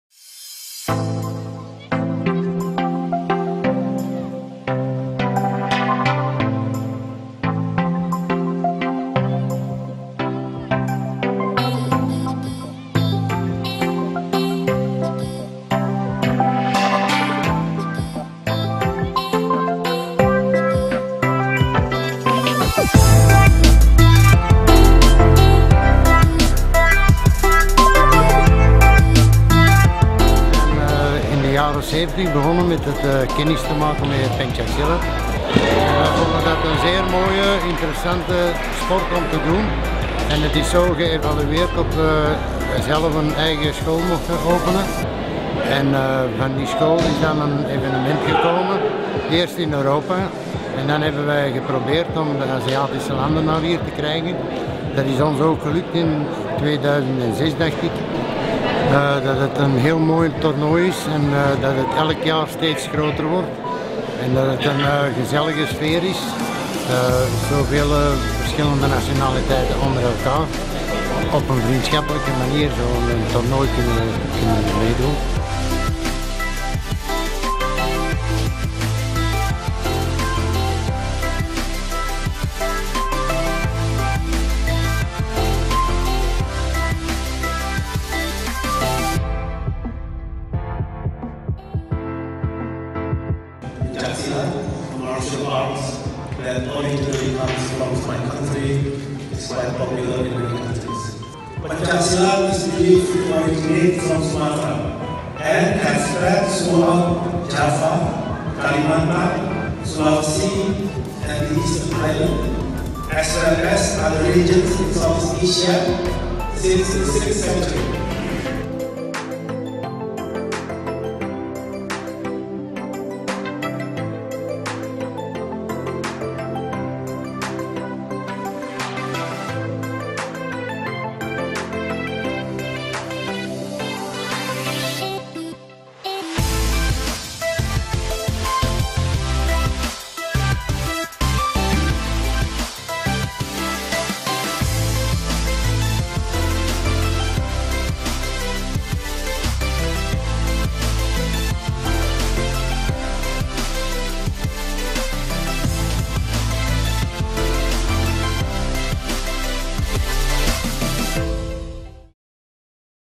International Pencak Silat Championship in Belgium
In Belgium there is an annual International Pencak Silat Championship organized by Pieters Family in Schoten. More than a hundred of athletes from the whole world compete every year.
Pencak Silat is a form of martial art originated from Indonesia.
Next to this wonderful championship, in Schoten there is also the Pencak Silat Sport School "Harimau" (Indonesian, meaning "tiger").